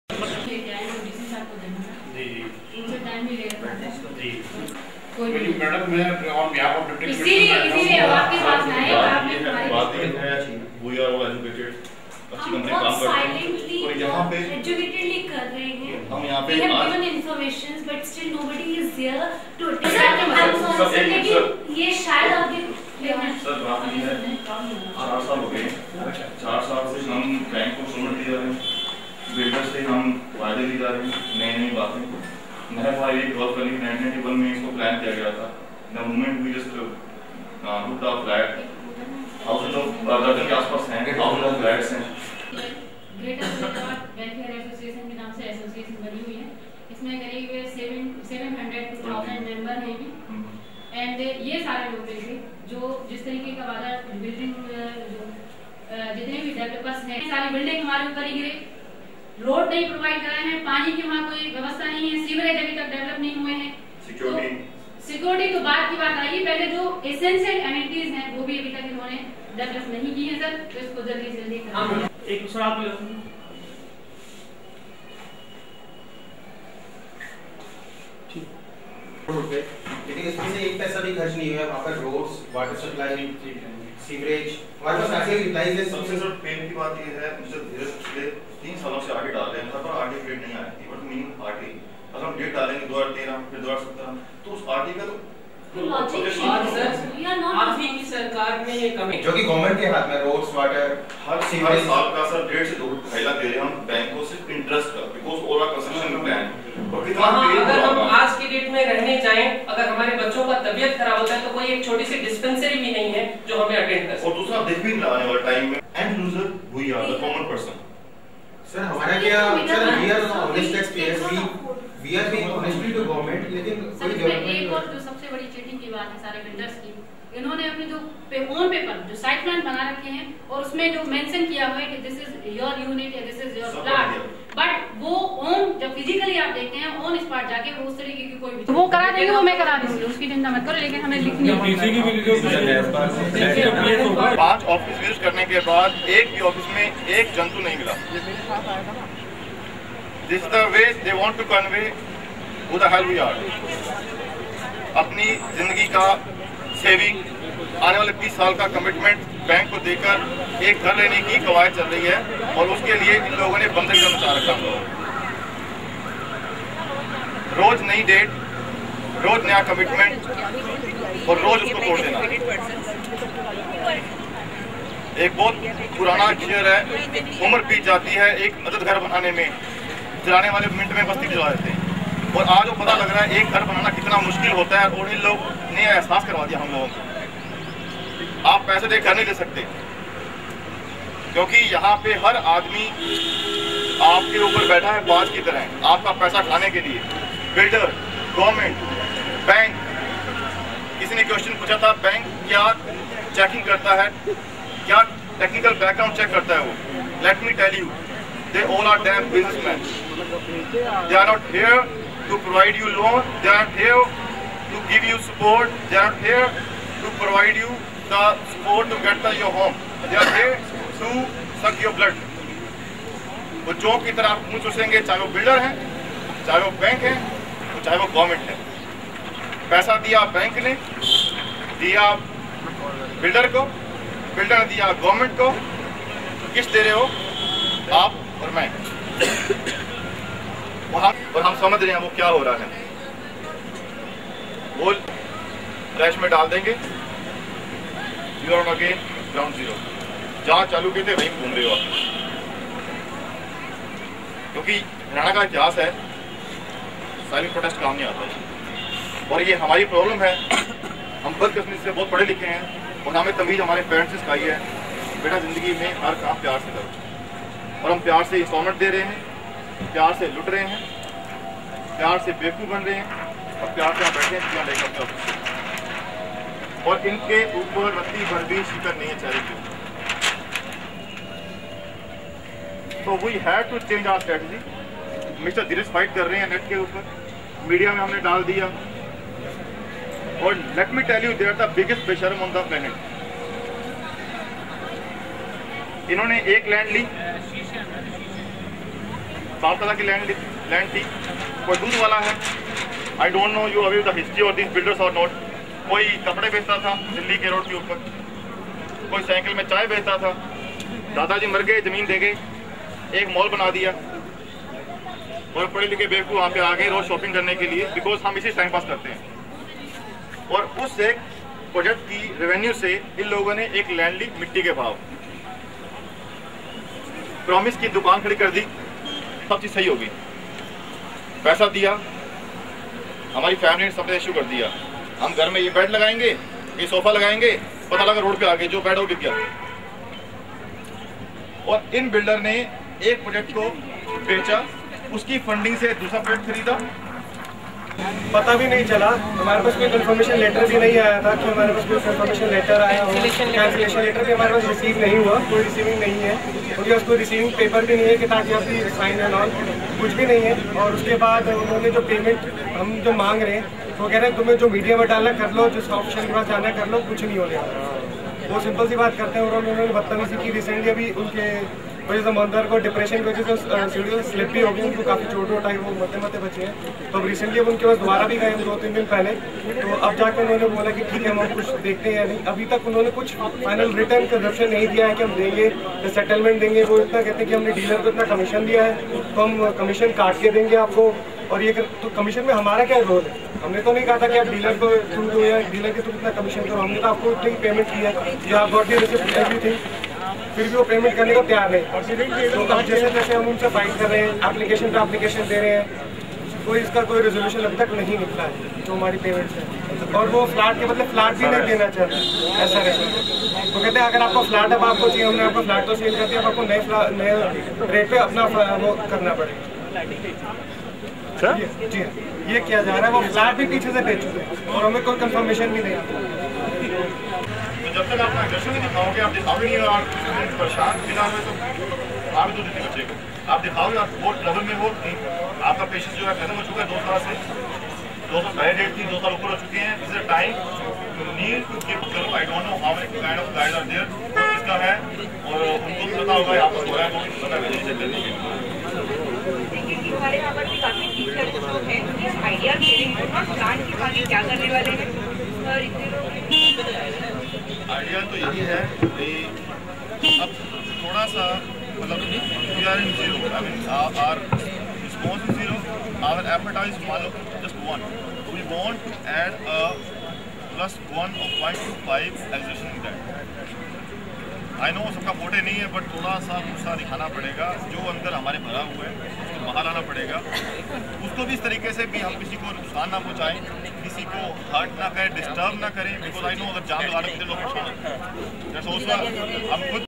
Protest against HUDA and Faridabad Neharpar Builders - Memorandum submission and Pankaj Speech
Memorandum submission and Pankaj Speech - Protest against HUDA and Faridabad Neharpar Builders Faridabad organized by Greater Faridabad Welfare Association - Morning Preparation

Investors and end-users of Greater Faridabad protested against HUDA and builders for lack of infrastructure development in Neharpar area and harassment by builders by various fraudulent activities.

Protest on 1st March 2011 outside HUDA Office, Sector 12, Faridabad